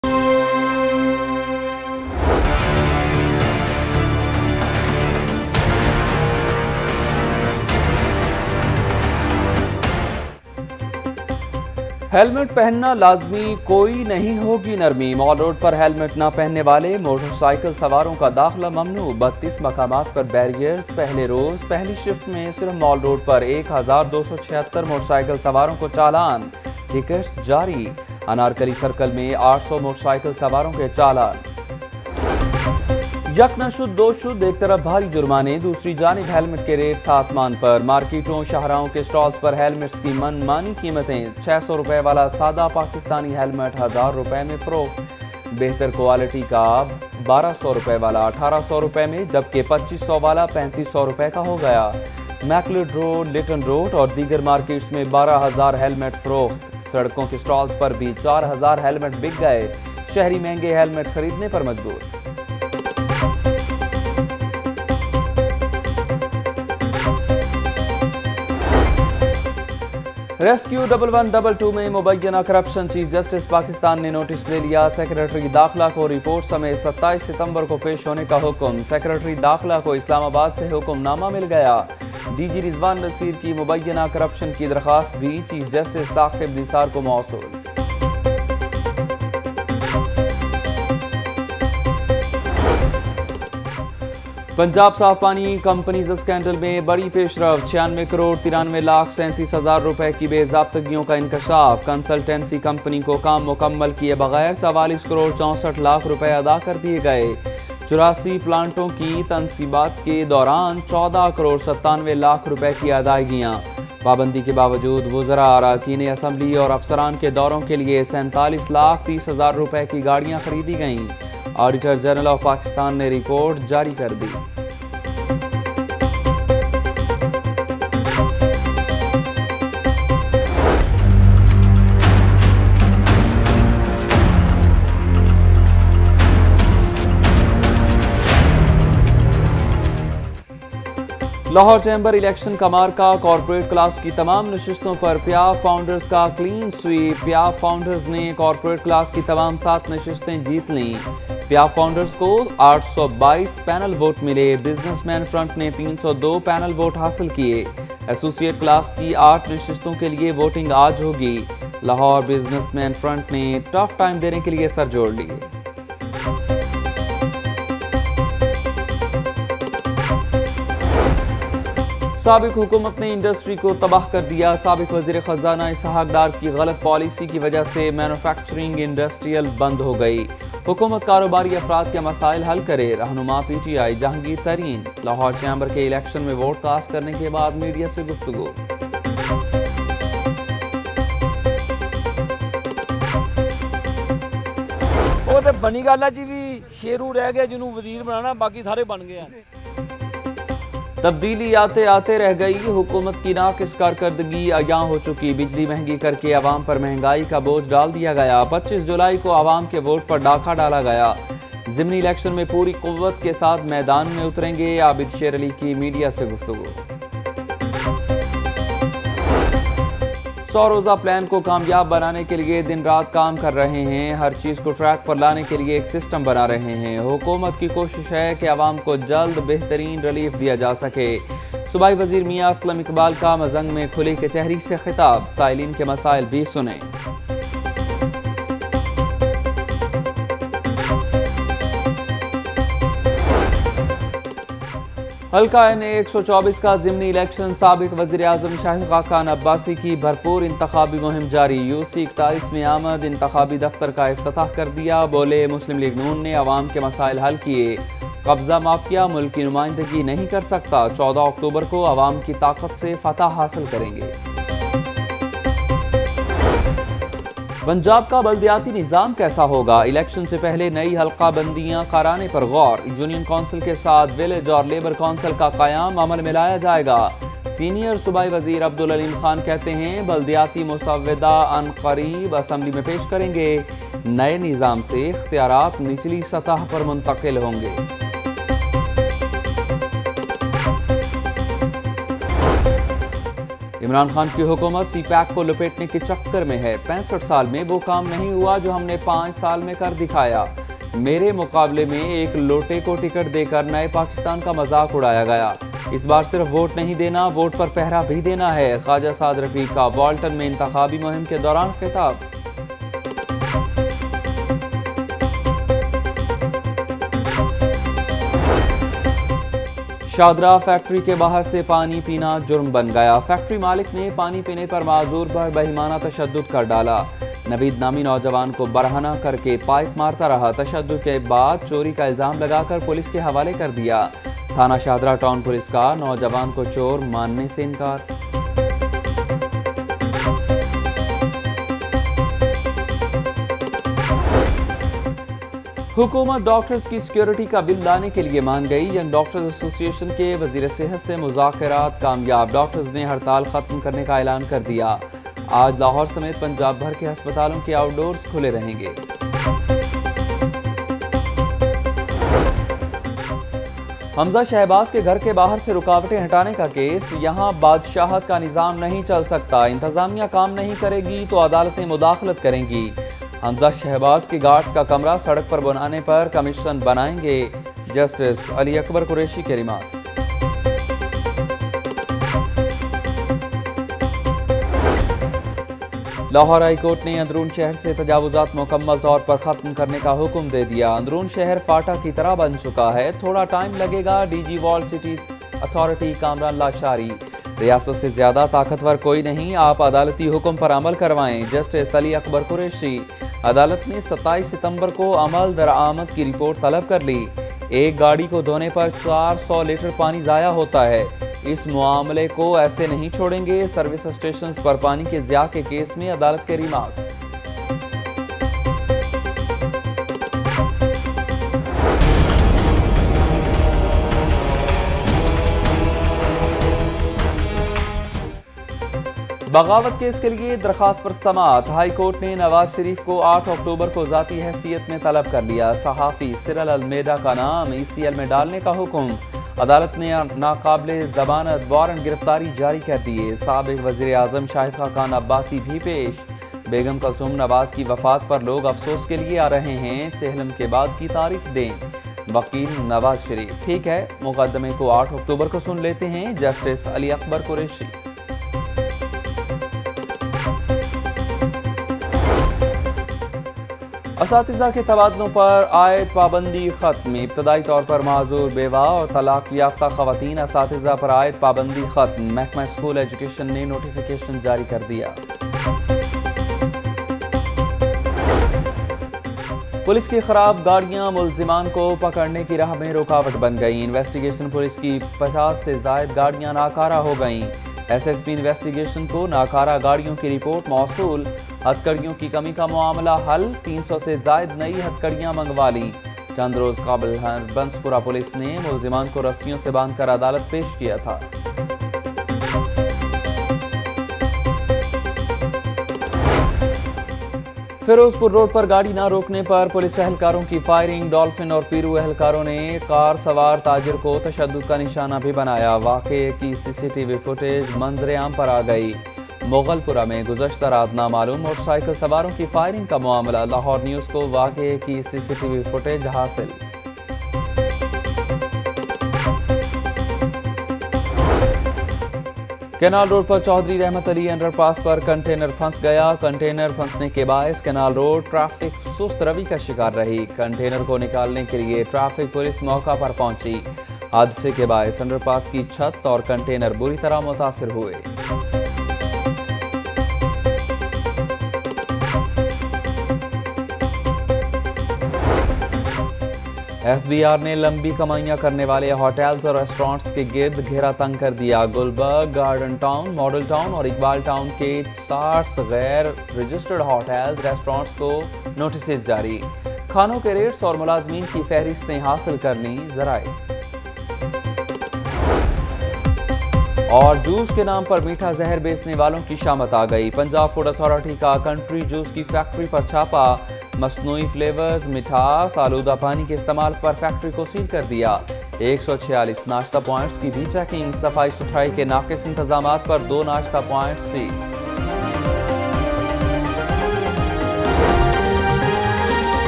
05 AM Headlines | Lahore News HD | 25 September 2018
Watch news for Young Doctors at strike again - India's Water terrorism continues - Weather overcast in Lahore - Meteorological department issues warning for more rains and floods - Thokar Niaz Beg: 16 year old boy falls off in canal and dies - New Pakistan means new mindset, PM Imran Khan addresses Govt. employees - Grieved over Imran Khan's response to India, says Shahid Khaqan Abbasi - India defeats Pakistan by 9 wickets in Asia Cup's super four match -  Medical colleges conducted entry tests for admissions - Increasing crime in city.